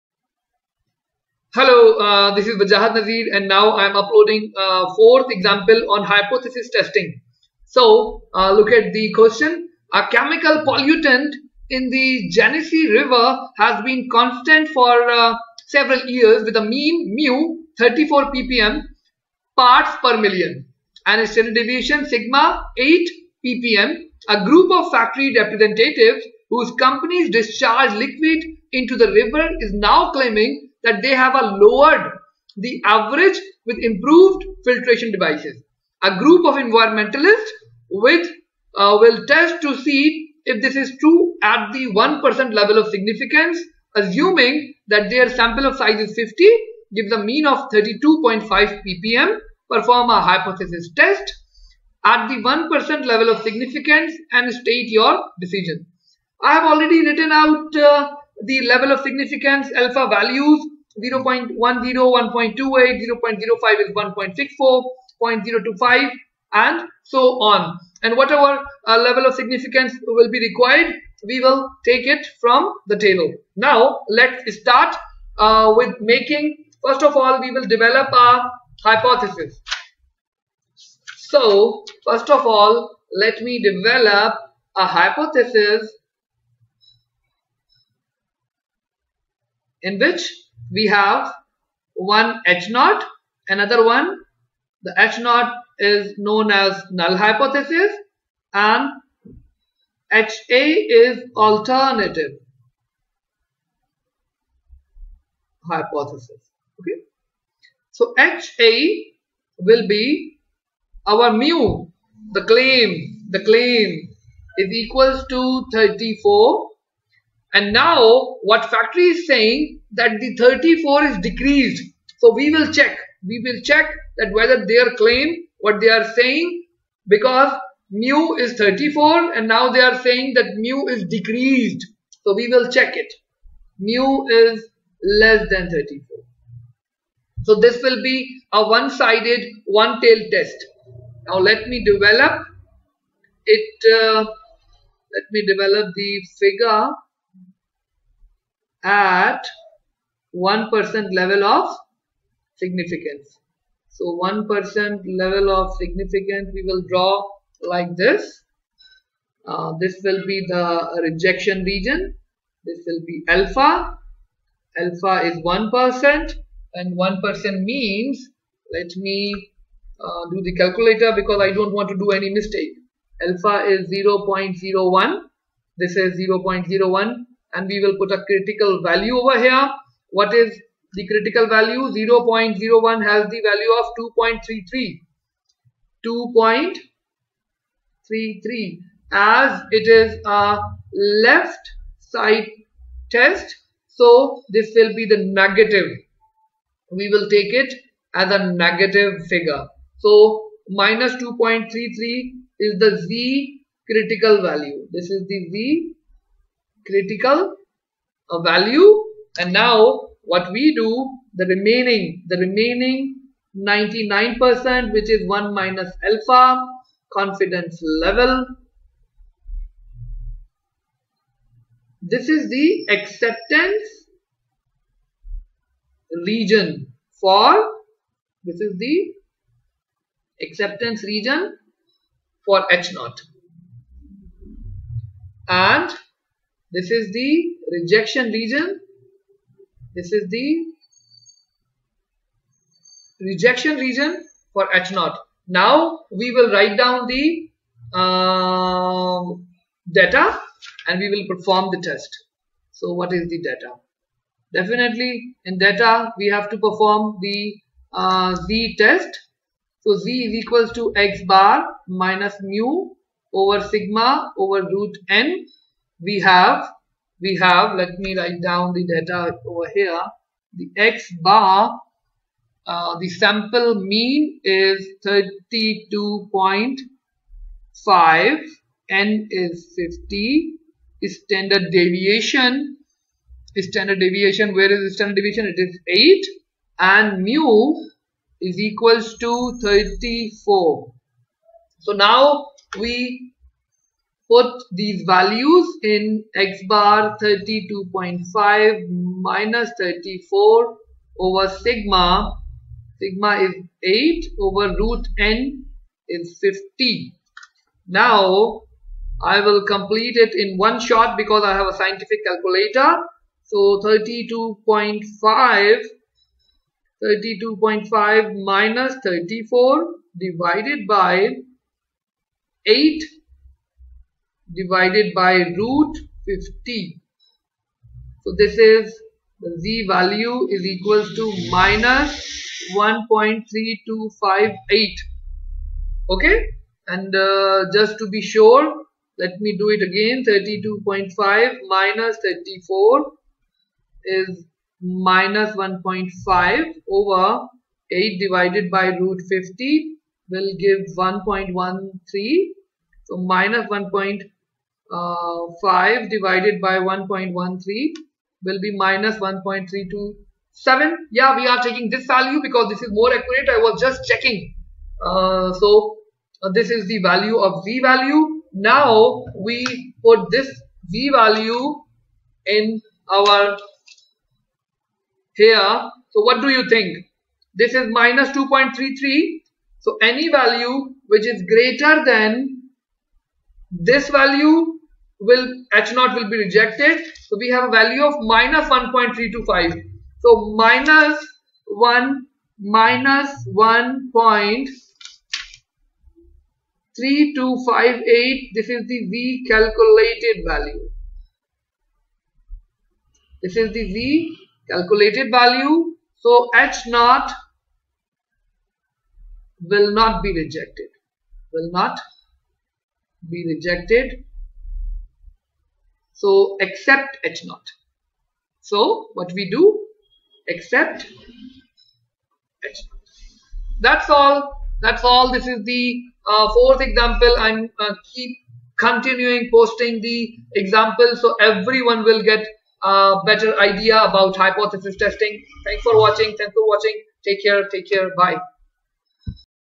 1.23.1 Hypothesis examples, Example # 4, Hypothesis Testing example, Hypothesis test, Sir Wajahat
A certain chemical pollutant in the Genesee River has been constant for several years with mean μ = 34 ppm (parts per million) and standard deviation σ = 8 ppm. A group of factory representatives whose companies discharge liquids into the river is now claiming that they have lowered the average with improved filtration devices. A group of environmentalists will test to see if this is true at the 1% level of significance. Assume that their sample of size 50 gives a mean of 32.5 ppm. Perform a hypothesis test at the 1% level of significance and state your decision.